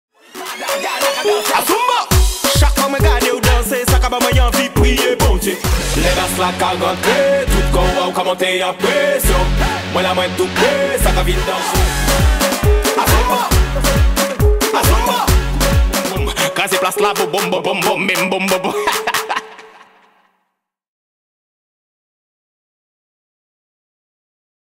Adelanto "AZUMBO" Jueves 8 de junio 2017 MundoGuyi
Estas clases se imparten en la ciudad de Madrid (España) en el Gimnasio “Club Atlas Fitness” en la Calle Estrella Polar 20.
Lunes y miércoles de 17:00 a 19:00 hrs.
Viernes de 18:30 a 19:30 hrs.